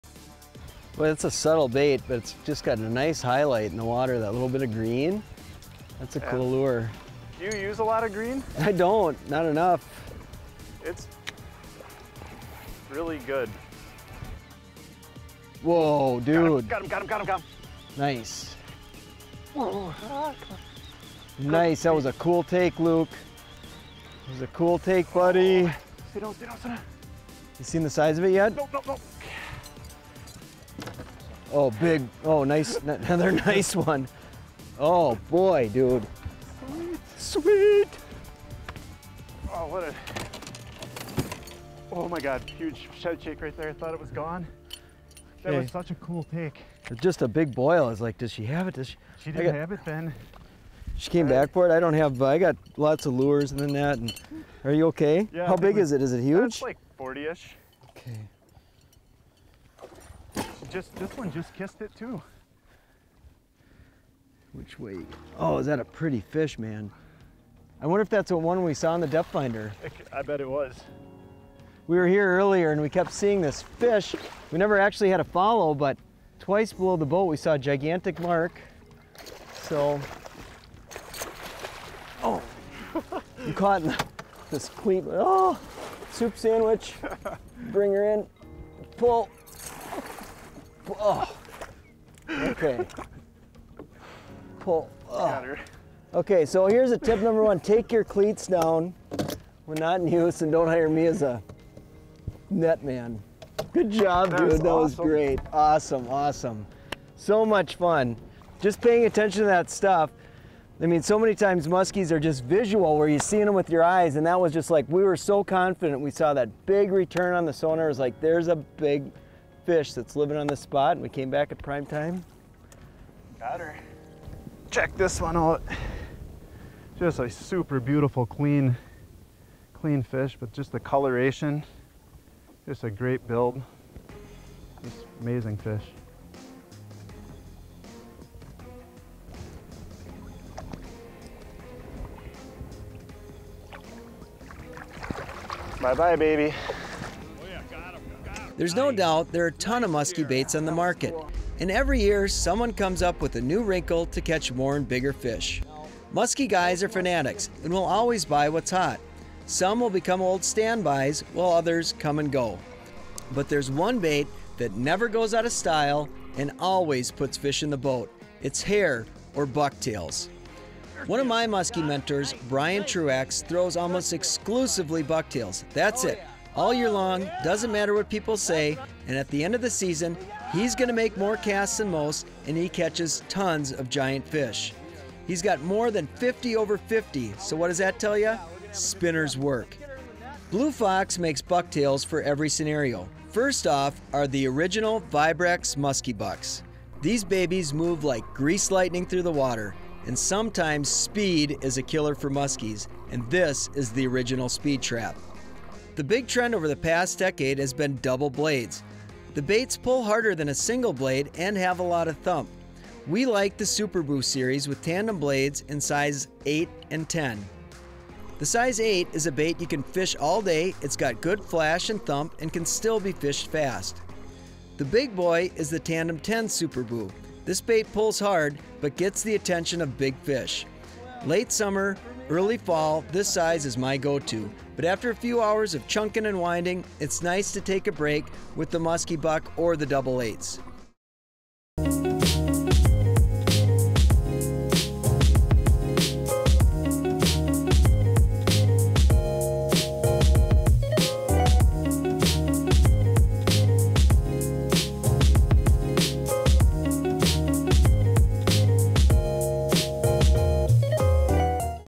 Great Musky Lures (ft. Jeremy Smith)
Jeremy Smith shares a few awesome musky lures that you really should try.

▬▬▬▬▬▬▬ MORE LINKS ▬▬▬▬▬▬▬